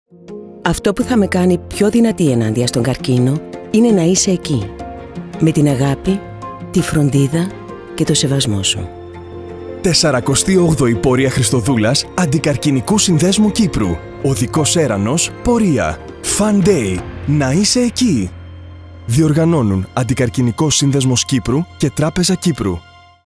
Πορεία Χριστοδούλας 2023
Να είμαστε όλοι εκεί και φέτος, δίπλα στον Αντικαρκινικό Σύνδεσμο Κύπρου, με αγάπη και σεβασμό για κάθε άτομο με εμπειρία καρκίνου.
48η Πορεία Χριστοδούλας Αντικαρκινικού Συνδέσμου Κύπρου.
Fun Day Κυριακή 2 Απριλίου
Σε όλες τις πόλεις:
Οδικός έρανος Πέμπτη 6 Απριλίου
Πορεία Κυριακή 9 Απριλίου